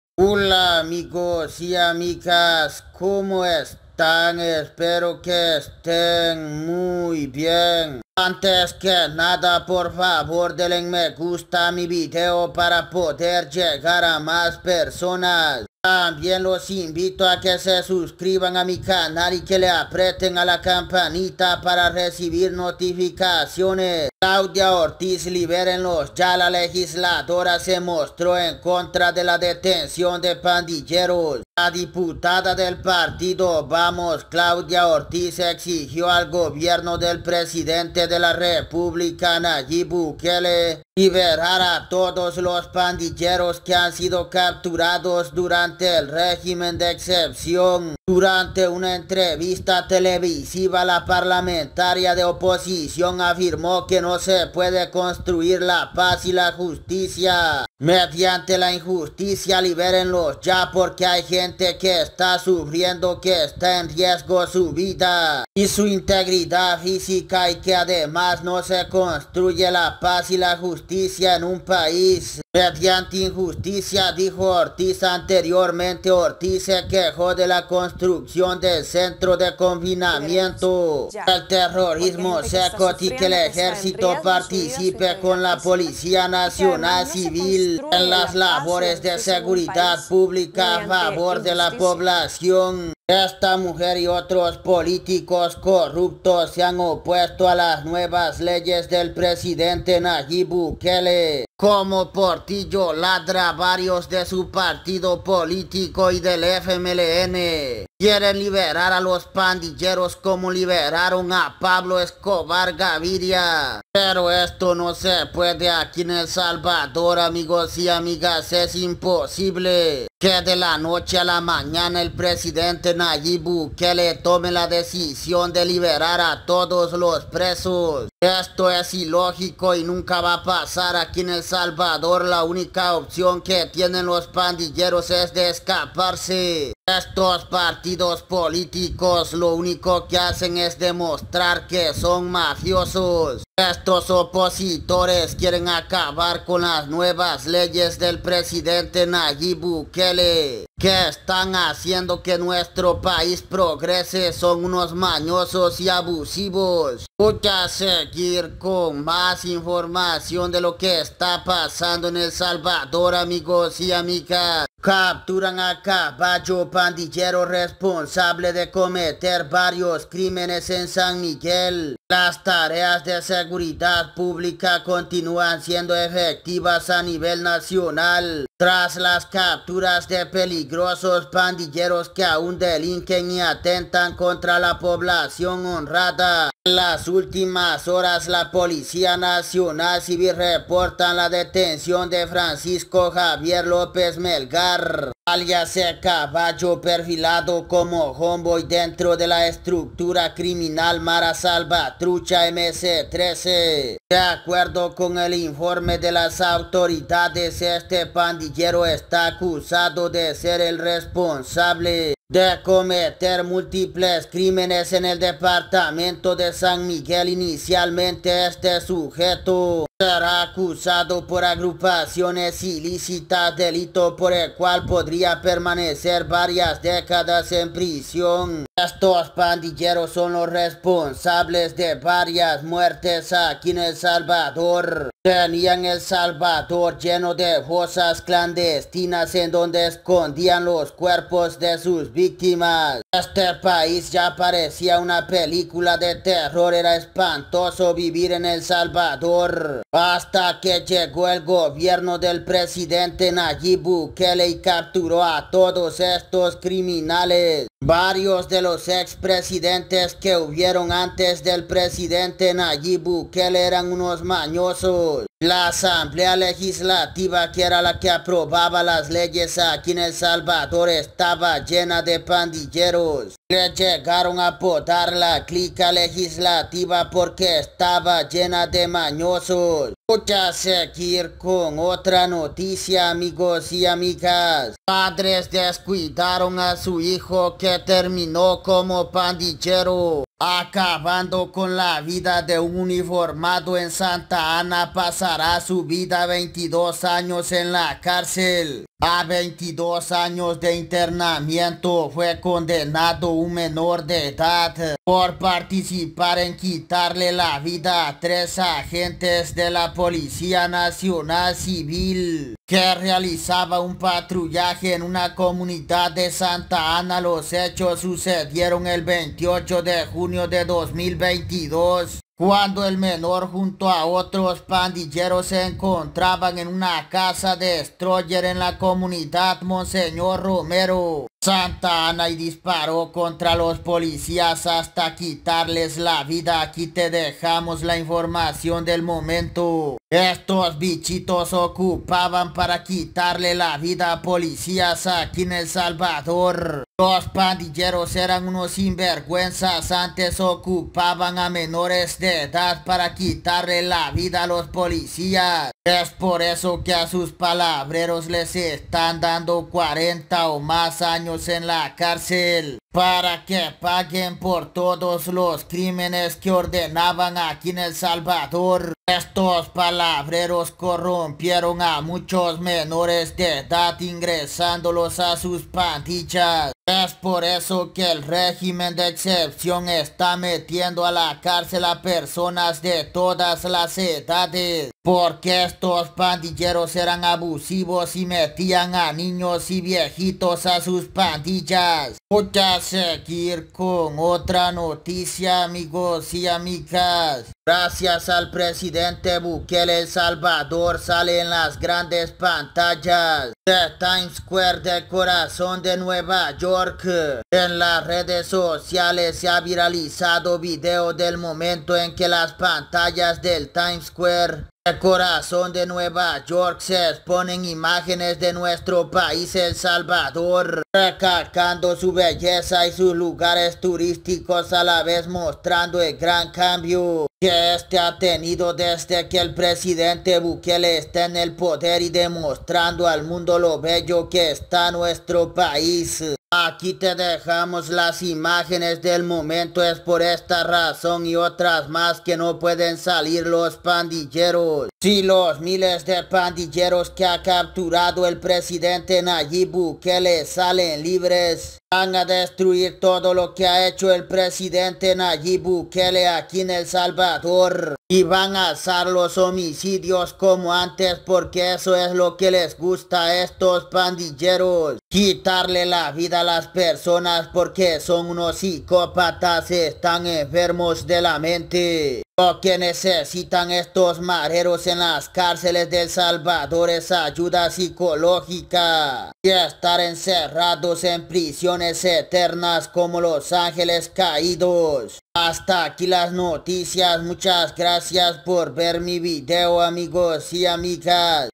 Claudia Ortiz: “¡Libérenlos ya!” capturan asesino en serie vichito pandillero mato policia BUKELE
Gracias al Presidente Bukele El Salvador sale en las grades pantallas de Times Square el corazón de Nueva York